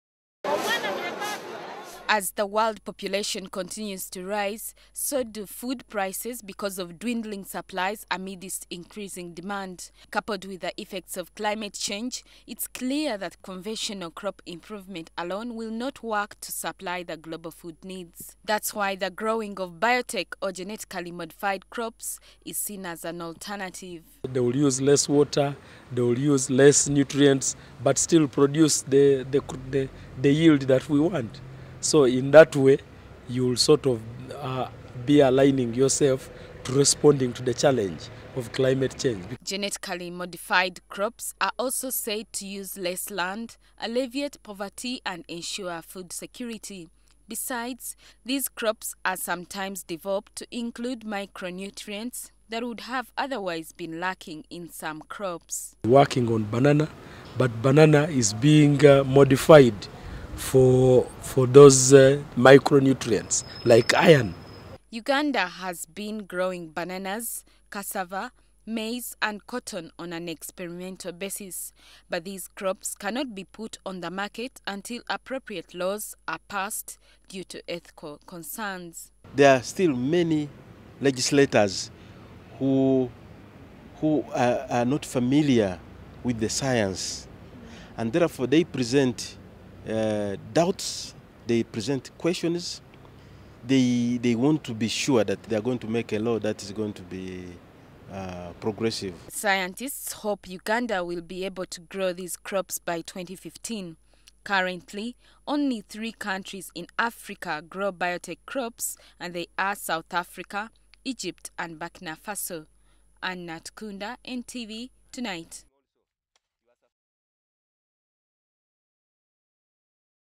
Global launch of GMOs
The 2010 Global Status report on Commercialized Biotech or Genetically Modified crops is out. The report launched simultaneously across the world shows that in the year 2010 accumulated hectarage genetically modified crops exceeded 1 billion, making biotech crops the fastest-adopted crop technology.